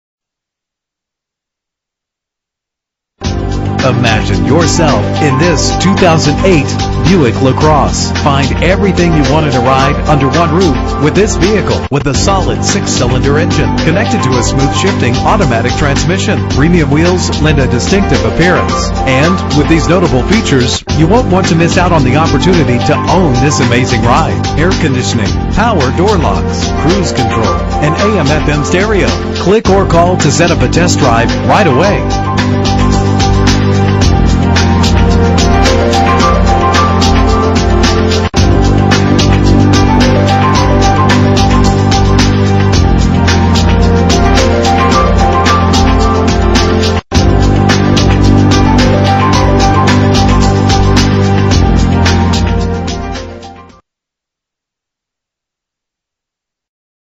2008 BUICK LACROSSE Sunrise Ford North Hollywood
Year: 2008
Make: BUICK
Model: LACROSSE
Trim: CXS
Engine: 3.6 liter 6 cylinder dohc
Transmission:
Color: blue
Mileage: 92794
Address:
5500 Lankershim Boulevard